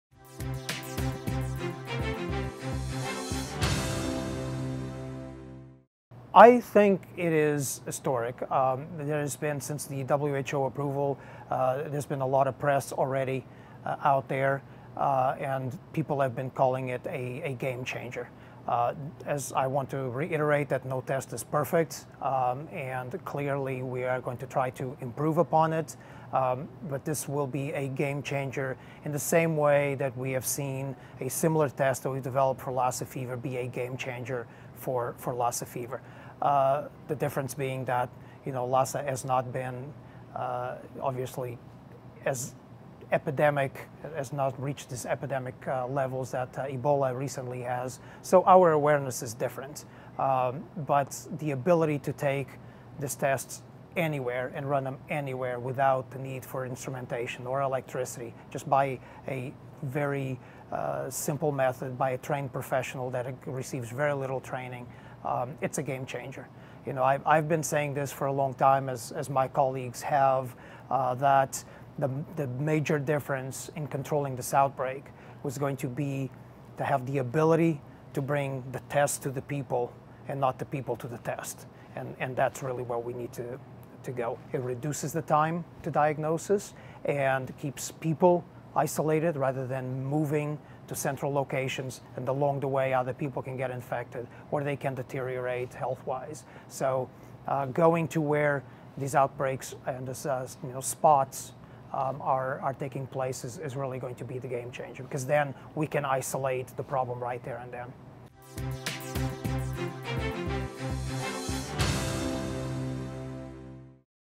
Ebola Diagnostic Test Key to Battling Ebola
In this MyMCMedia Extra video, Zalgen Labs’ co-founder Dr. Luis Branco says the rapid test to diagnose Ebola is a game changer when it comes to fighting the spread of this deadly virus. Take a look.